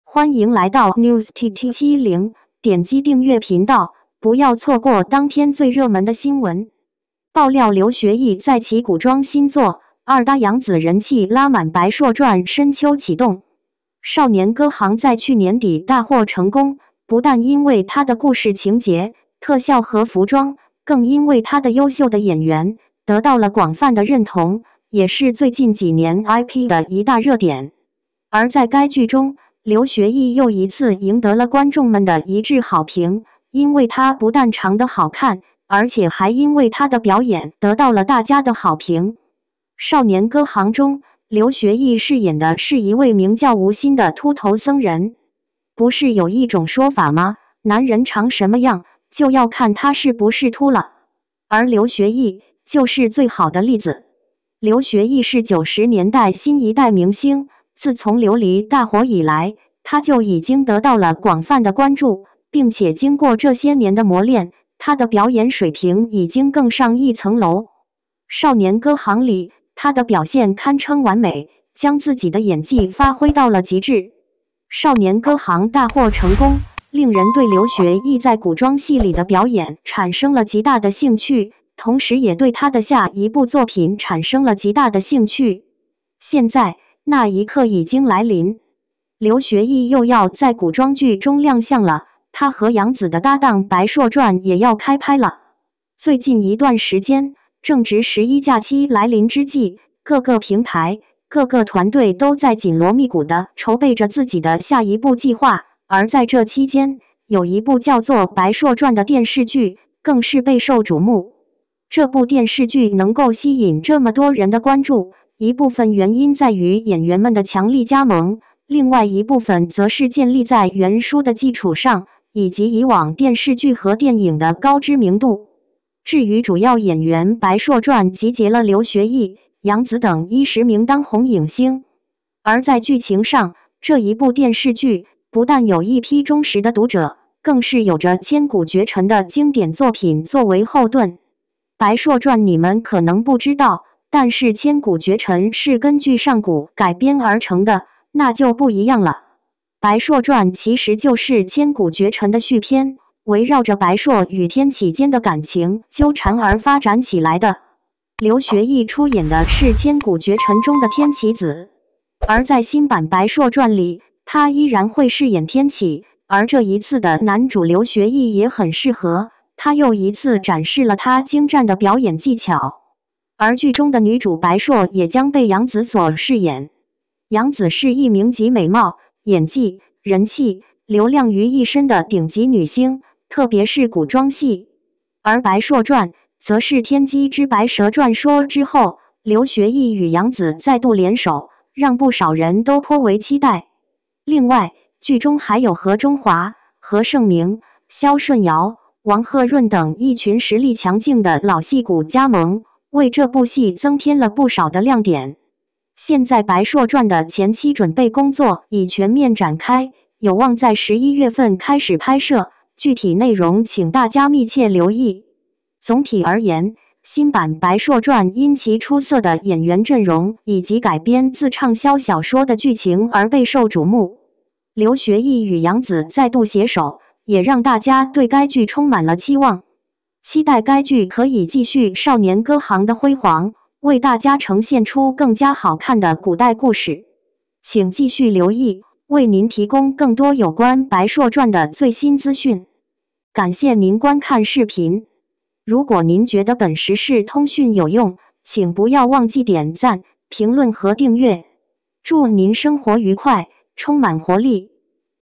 【NEWS TT7010月11日】爆料#刘学义 再启古装新作，二搭#杨紫人气拉满，《白烁传》深秋启动
爆料刘学义再启古装新作，二搭杨紫人气拉满，《白烁传》深秋启动